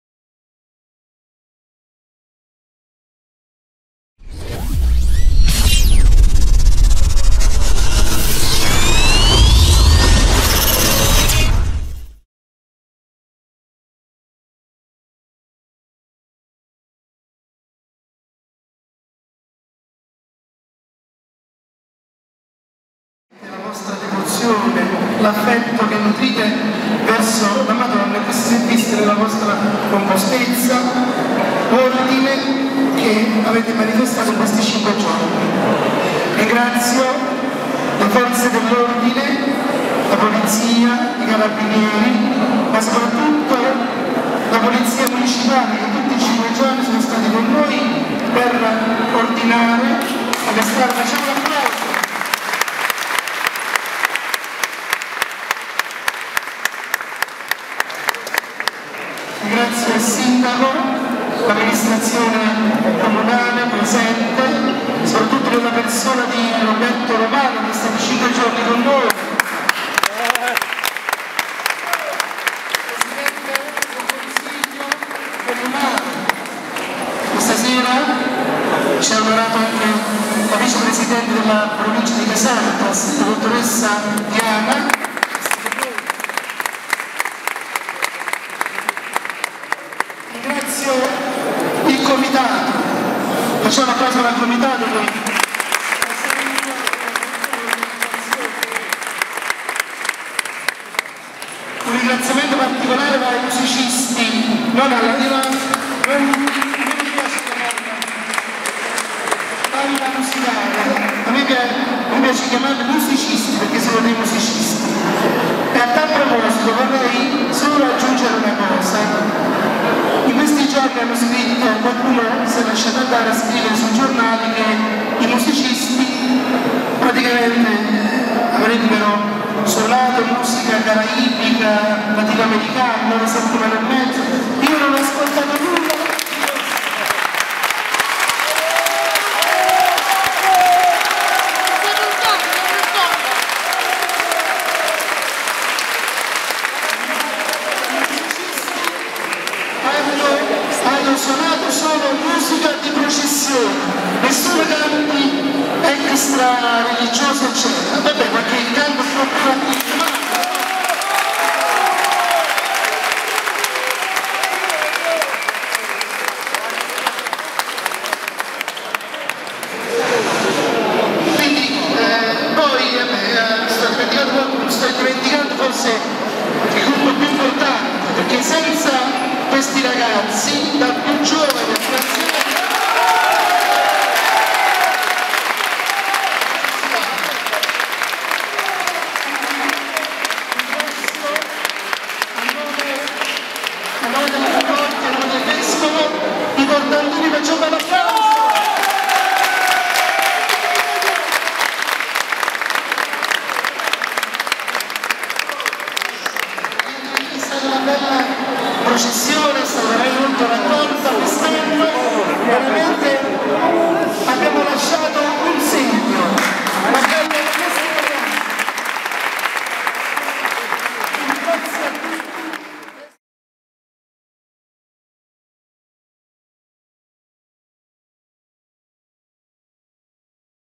discorso del parroco don Antonio Fabozzi al rientro della processione della Madonna di Casaluce
Dopo il rientro della Madonna di Casaluce nella Chiesa Santi Filippo e Giacomo di Aversa il parroco don Antonio Fabozzi si complimenta con tutti per la buona riuscita della processione.